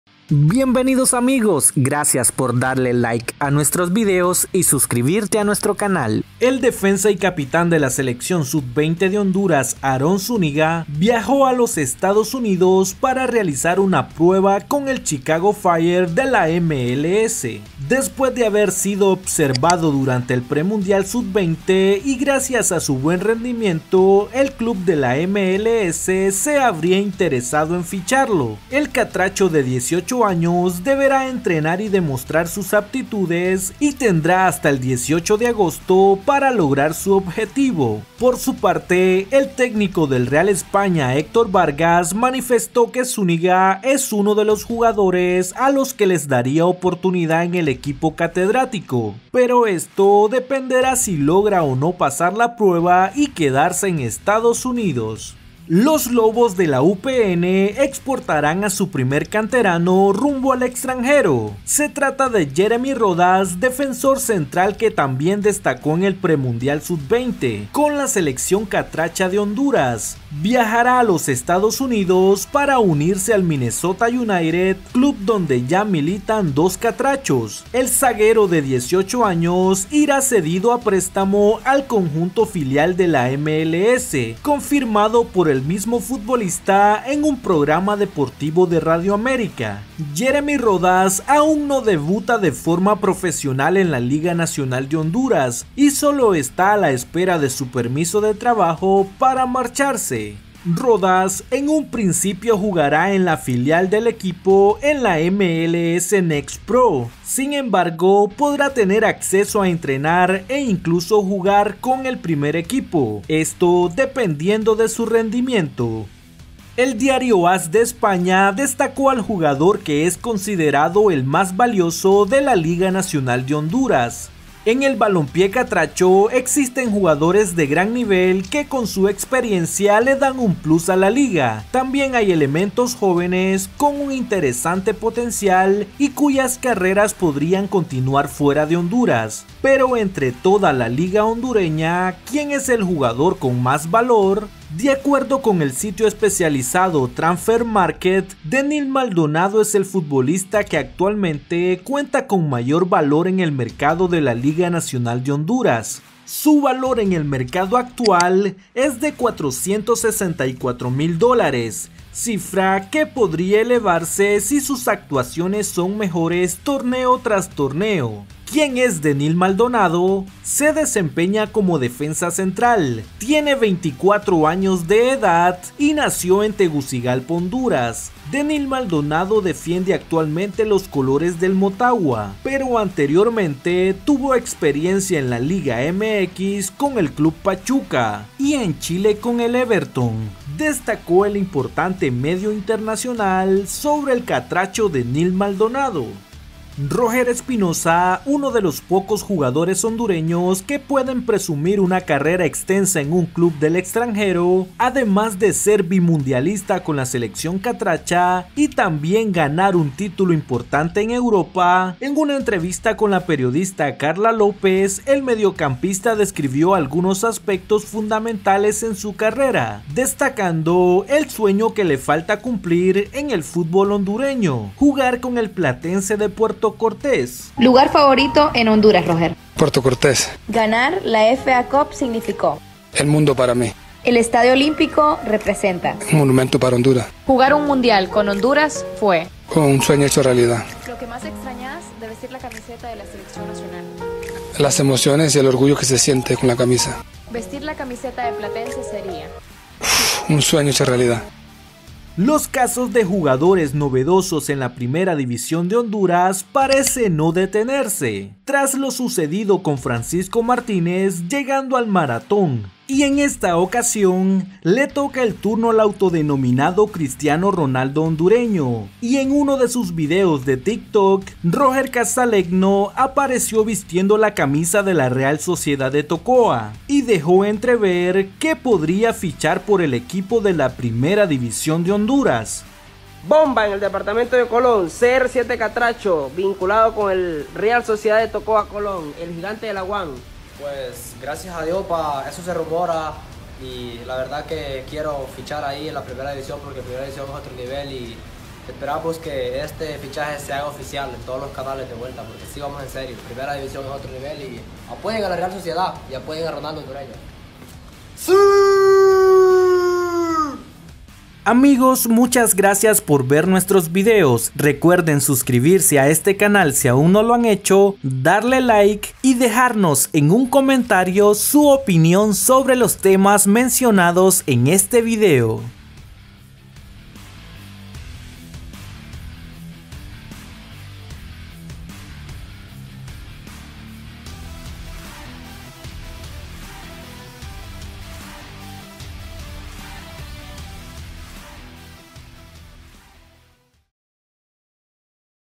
🚨 GEREMY RODAS NUEVO LEGIONARIO 🇭🇳 | DENIL MALDONADO EL JUGADOR MAS CARO💰 | CR7 HONDUREÑO A PRIMERA🔥
🚨 GEREMY RODAS NUEVO LEGIONARIO 🇭🇳 | DENIL MALDONADO EL JUGADOR MAS CARO 💰 | CR7 HONDUREÑO A PRIMERA 🔥 | ROGER ESPINOZA Y SU SUEÑO PENDIENTE 🦈